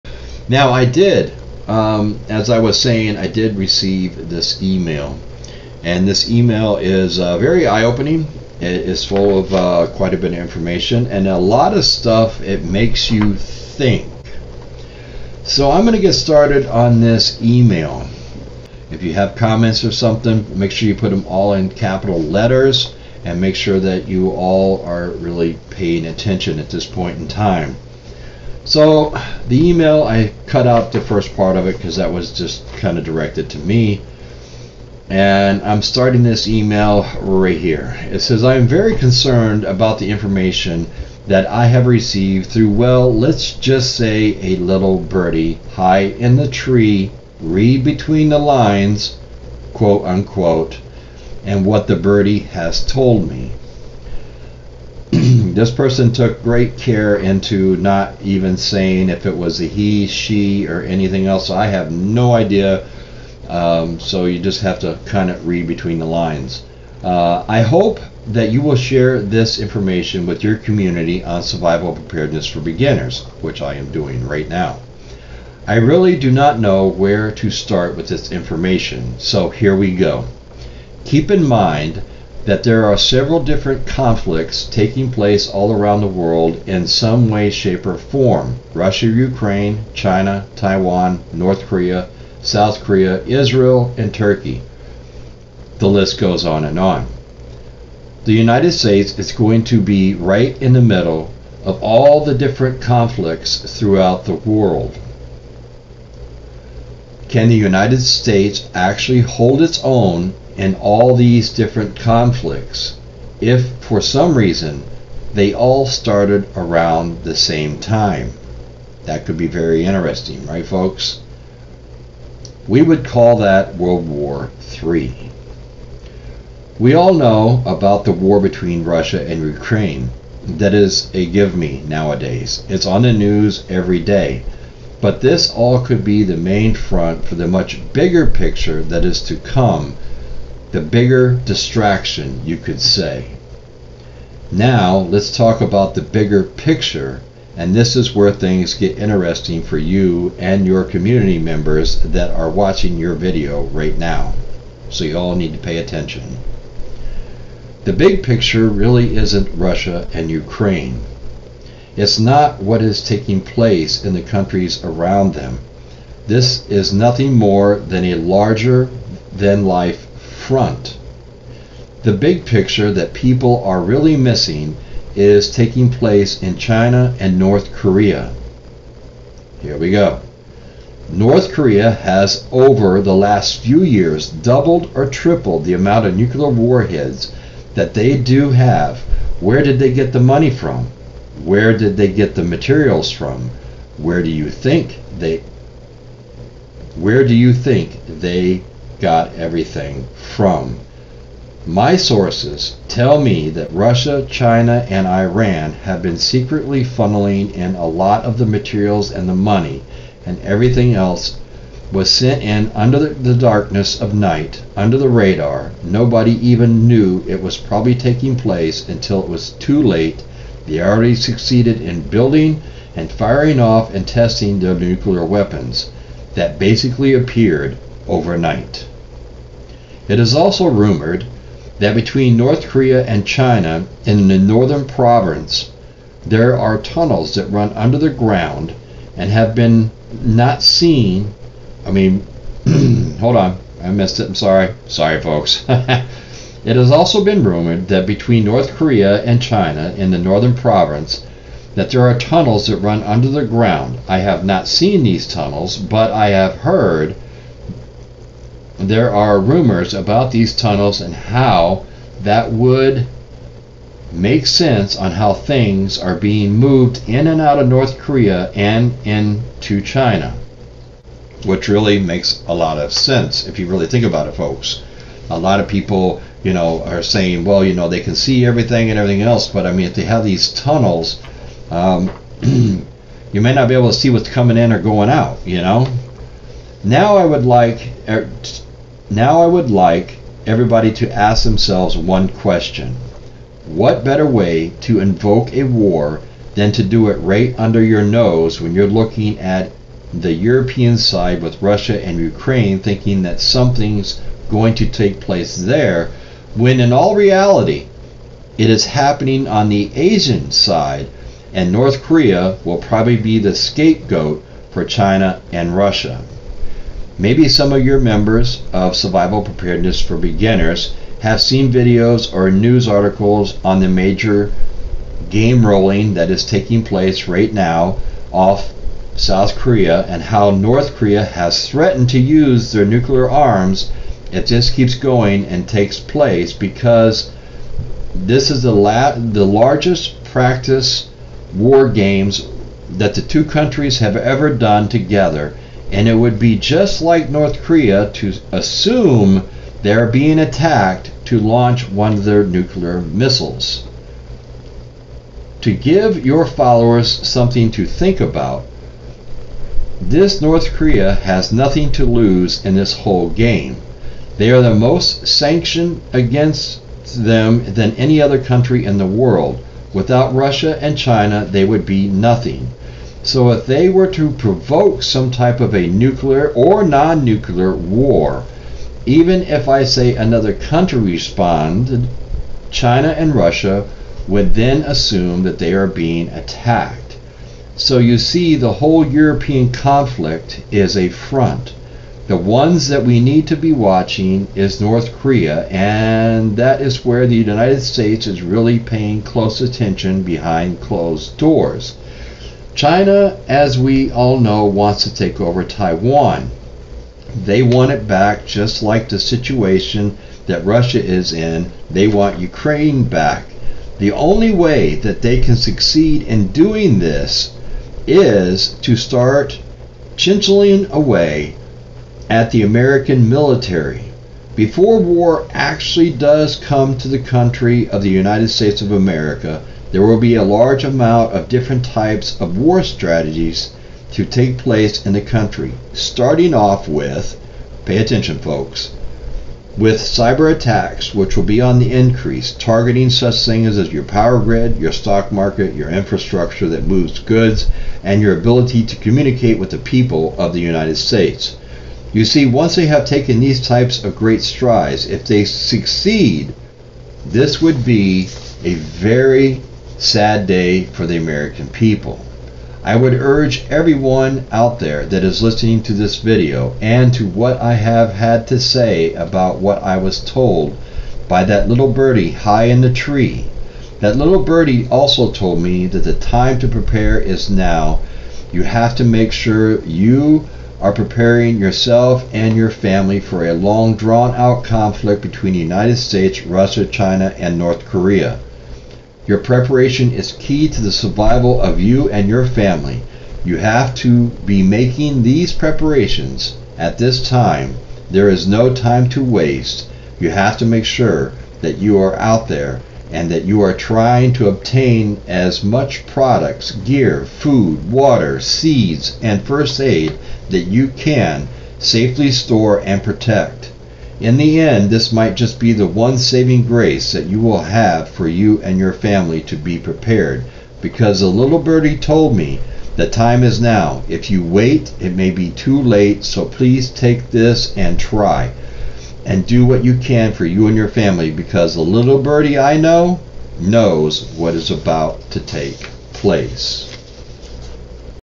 Shocking Inside Information Uncovered: Fact or Fiction
In a world filled with secrets, conspiracies, and hidden agendas, how can we tell if the inside information we come across is true or false? Join me in this intriguing and thought-provoking video as we dive into inside information I received. We'll examine some of the most shocking and controversial pieces of inside intel,  that I have received. Discover the meat of the information and Uncover the truth and become a more discerning consumer of information as we journey through the fascinating world of hidden knowledge. Don't miss this captivating video.